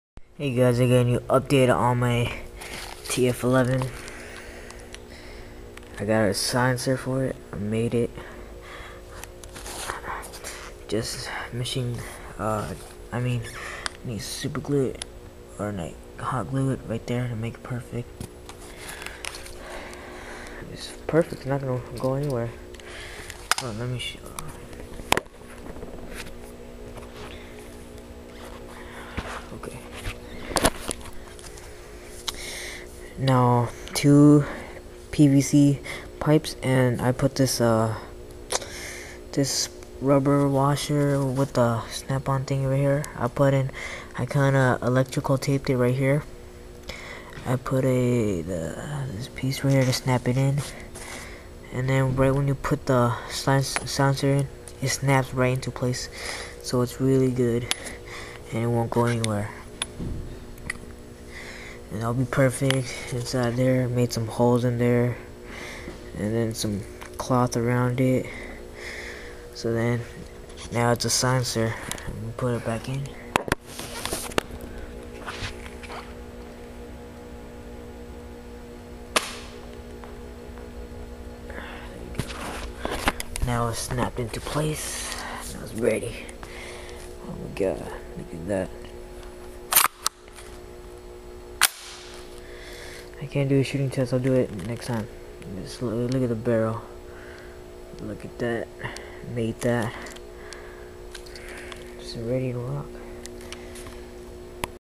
TF11 WITH SILENCER
MADE THIS SILENCER
HOW TO MAKE THIS SILENCER WILL BE COMING SOON
SILENCER IS ACCURATE GREAT AND WONT DROP OR FALL IT FITS SNUGLY